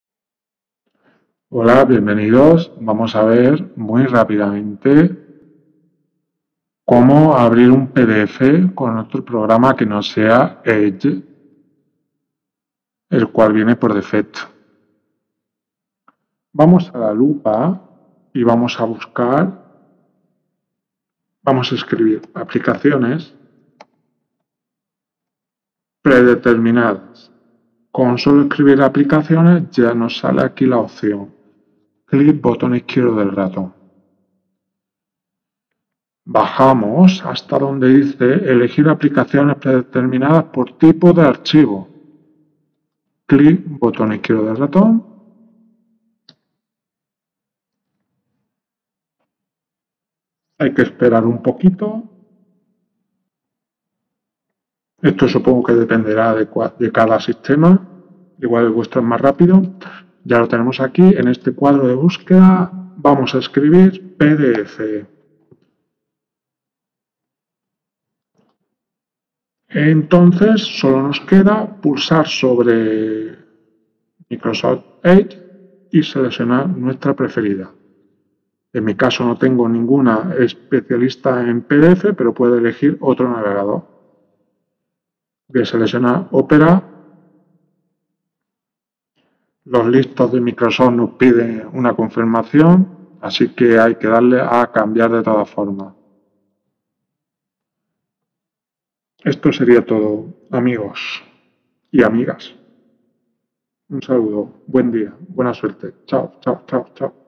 Abrir PDF con otro programa que no sea EDGE
Por defecto, windows 10 configura el sistema operativo para que abra todos los archivos pdf con su navegador Microsoft Edge. En el vídeo enseño como cambiarlo. Es muy fácil, ve a aplicaciones predeterminadas, luego establecer por tipo de archivo y finalmente selecciona la aplicación o programa que más te guste para leer un pdf